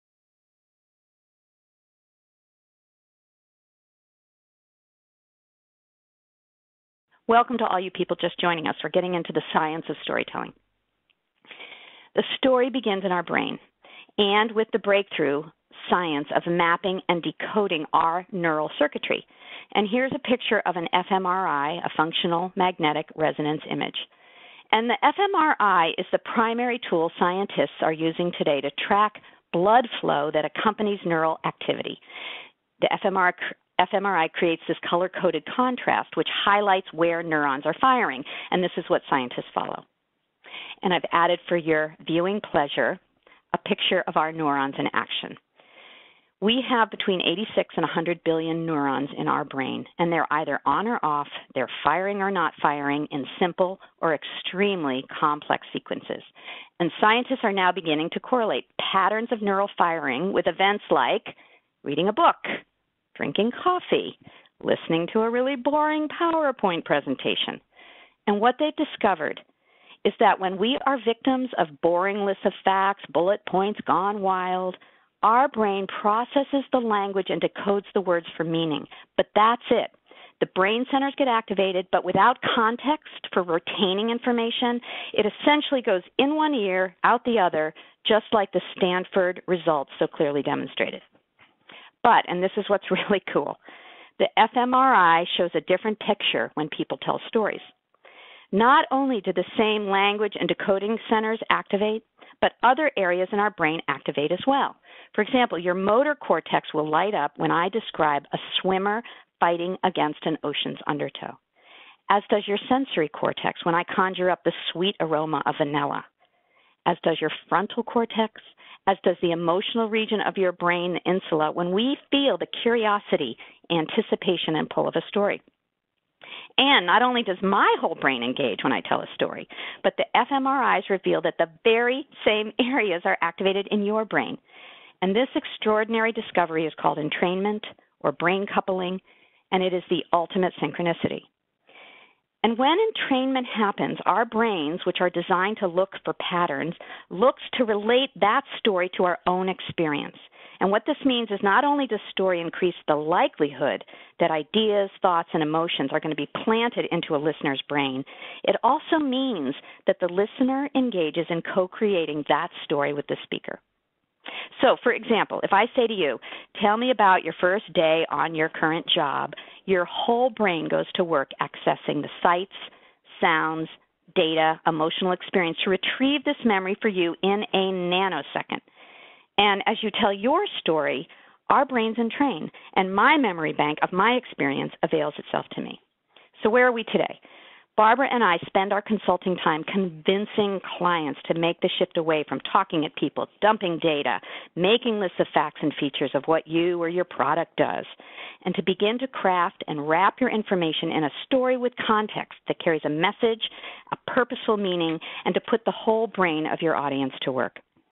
Neuroscience of Business Storytelling
How brain coupling happens and how it helps us connect rather than merely present.